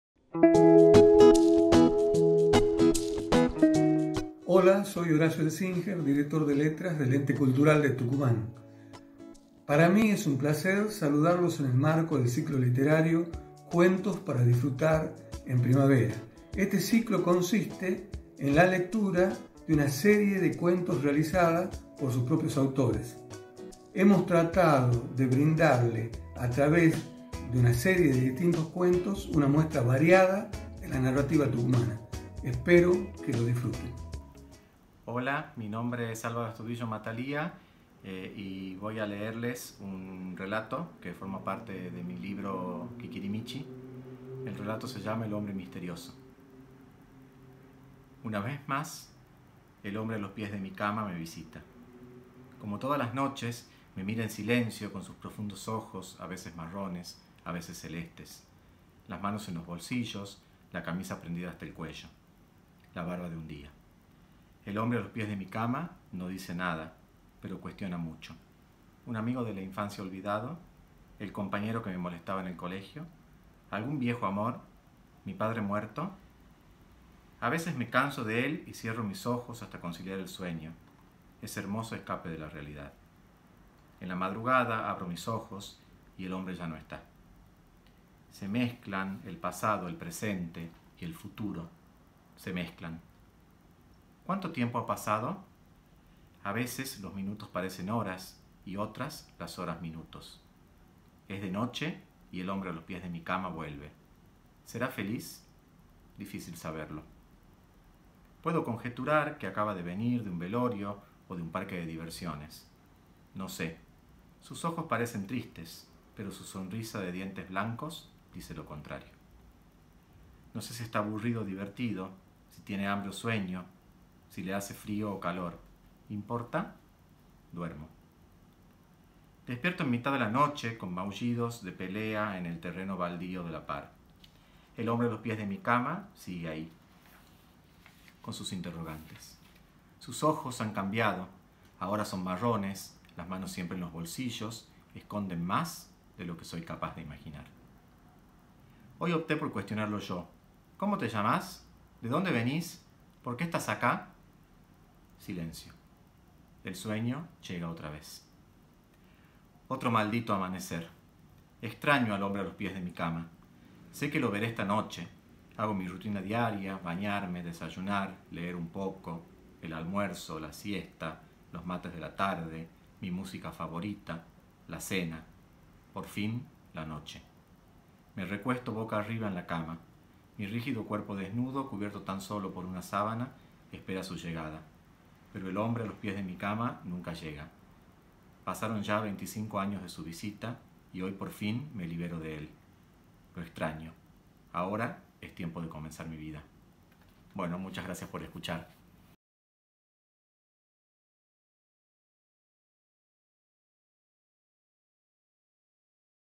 Álvaro Astudillo | Cuentos para disfrutar en primavera
Cuentos para disfrutar en primavera 📖🌸
Autores tucumanos comparten, desde la intimidad de sus hogares, trabajos de su propia autoría.